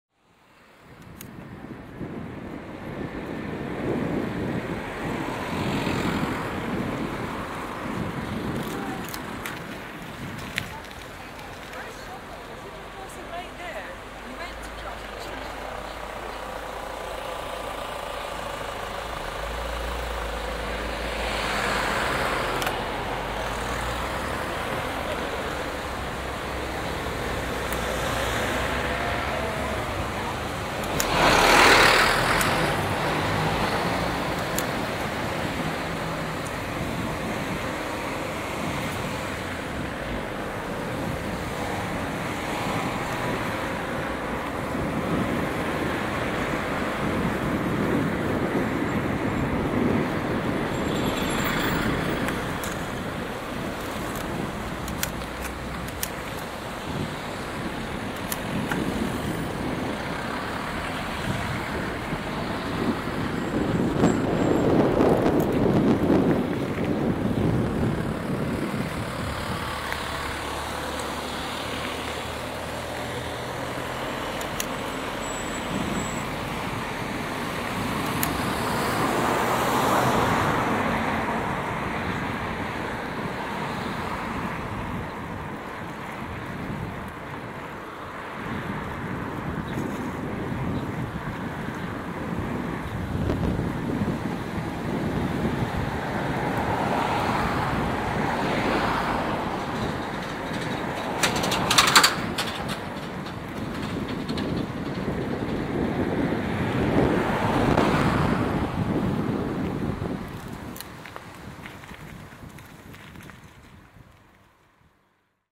Aviemore Traffic - Binaural Recording
Listen with headphones for the full effect. Cycling along Aviemore high street during an exceptionally busy time of year. The panorama images are from a quieter time.